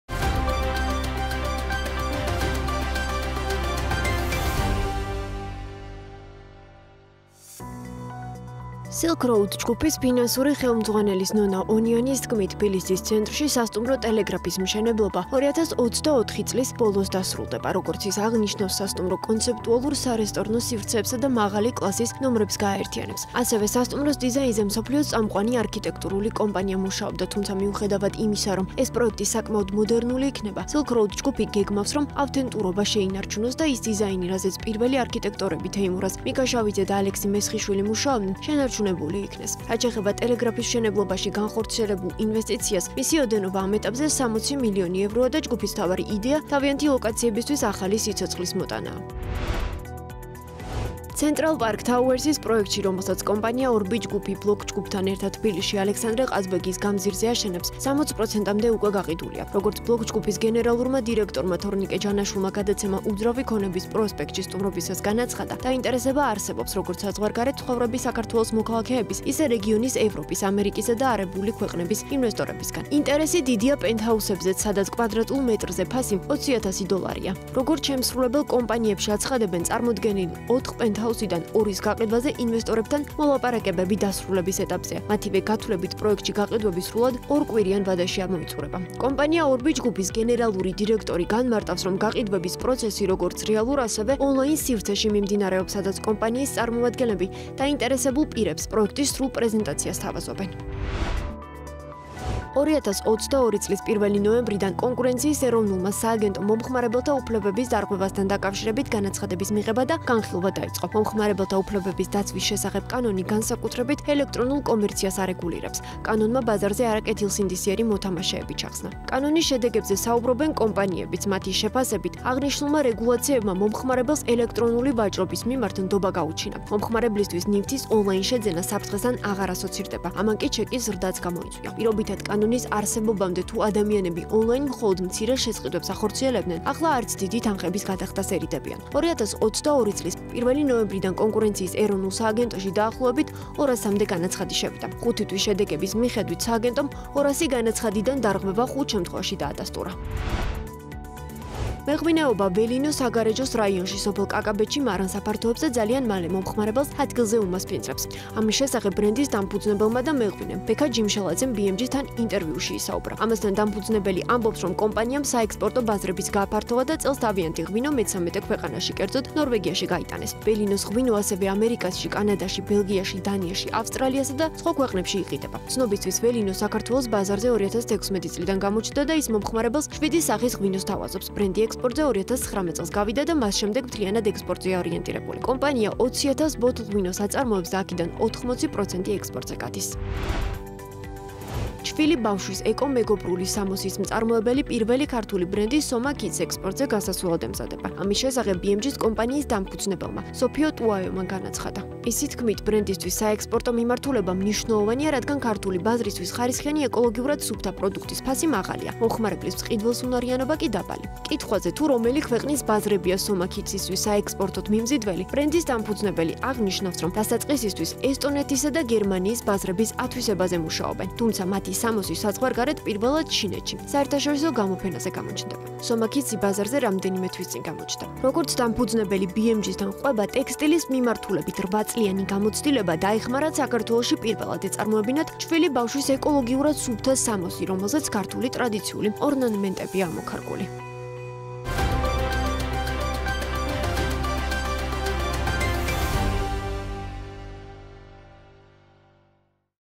ბიზნესსიახლეები bm.ge-სგან / 26.04.2023
„60 მლნ ევროს ღირებულების სასტუმრო „ტელეგრაფის“ მშენებლობა 2024 წლის ბოლოს დასრულდება“ - „სილქ როუდ ჯგუფი“;
„პირველადი შენატანი $180 000-ია“ - რა პირობით შეიძენთ "ჰოტელ ჰაბში“ 18-ნომრიან სასტუმროს?
რა შეცვალა მომხმარებელთა უფლებების დაცვის შესახებ კანონმა ბიზნესებისთვის?
"ვამატებთ სადეგუსტაციო და სარესტორნო სივრცეს" – „ველინო“;
„ქართულ ბაზარზე ჩვენი პროდუქტის მომხმარებელი მხოლოდ 20%-ია“ – „სომა ქიდსი“ ექსპორტზე გასასვლელად ემზადება;